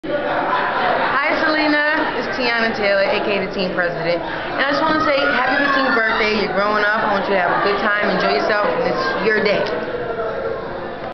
TEYANA TAYLOR WISHES SELENA A HAPPY BIRTHDAY
HAPPY BIRTHDAY 15TH SELENA